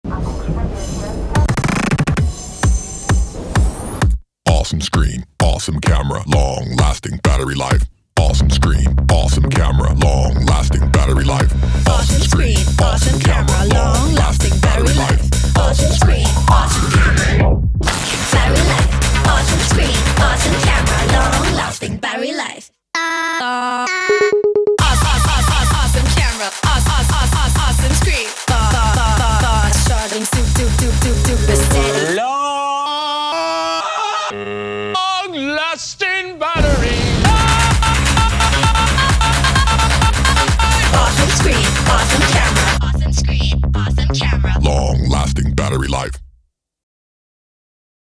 Galaxy A Series: Awesome is for everyone
Awesome is for everyone with the all new Galaxy A series. Awesome screen, awesome camera, long lasting battery life!